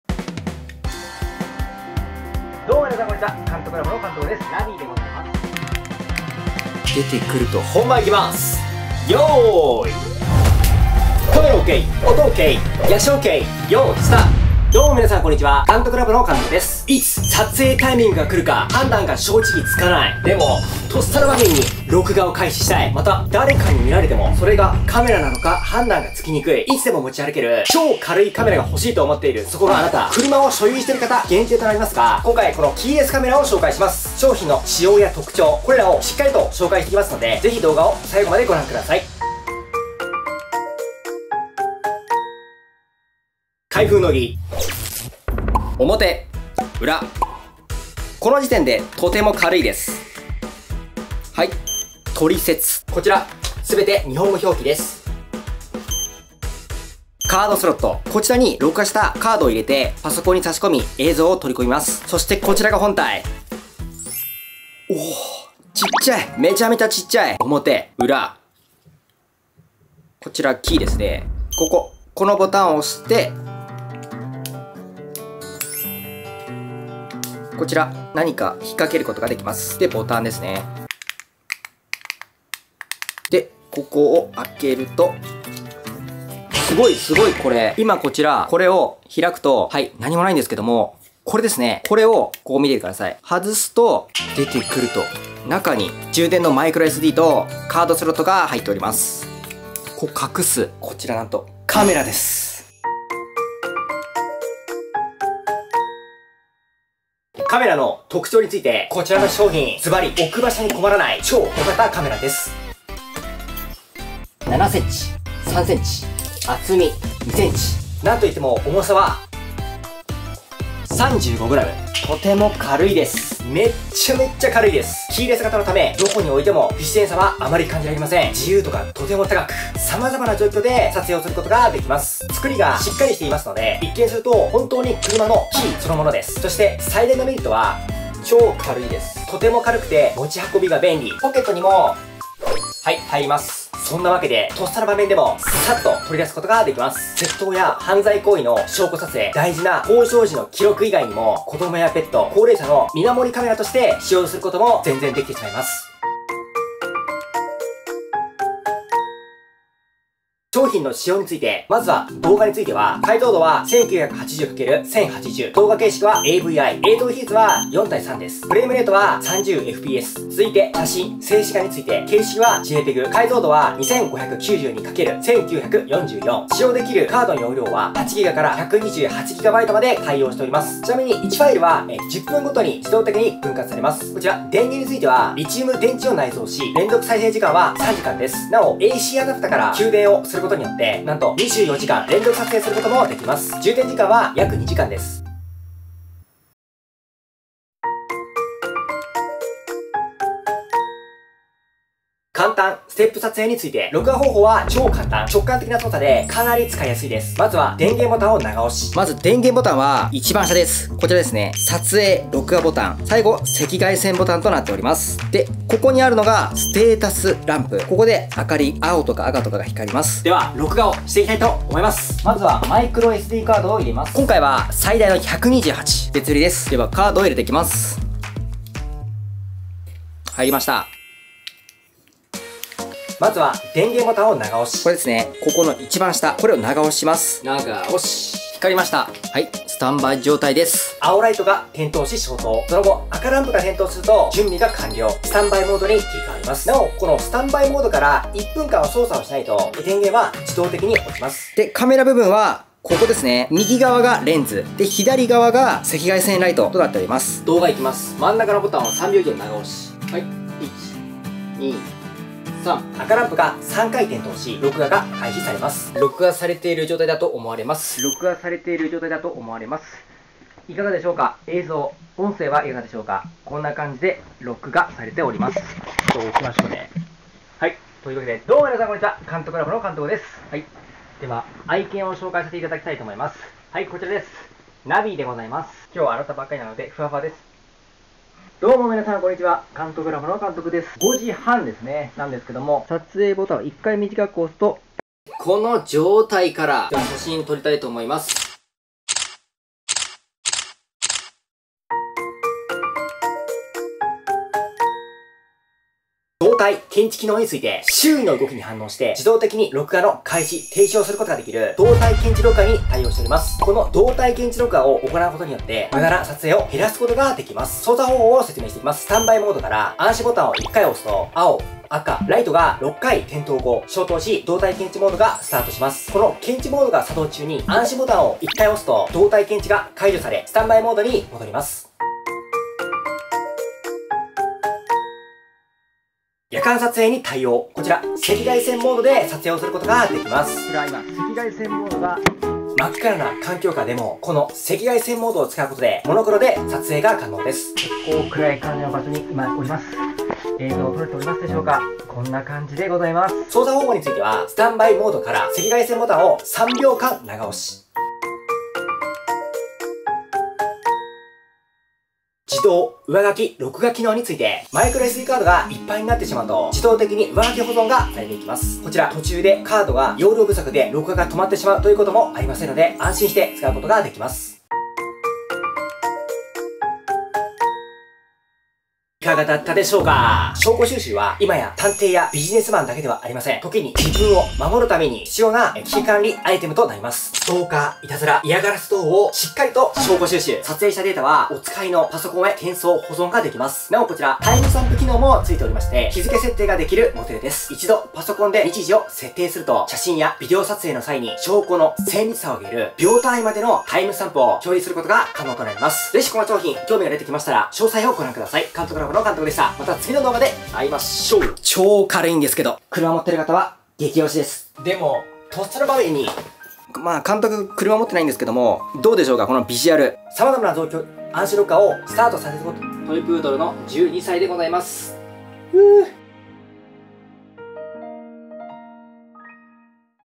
【キーレス擬態型】普段持ち歩いても問題なし、急な撮影タイミングに即座に反応できるキーレス型小型カメラをご紹介します【PONAOO】
Amazon商品ページ（キーレス型カメラ / PONAOO）

・証拠収集は、今や探偵やビジネスマンだけではありません。
時に自分を守るために必要な危機管理アイテムです。

いつも持ち歩ける軽いカメラが欲しい！と思っている方におすすめしたいキーレスカメラを今回ご紹介します。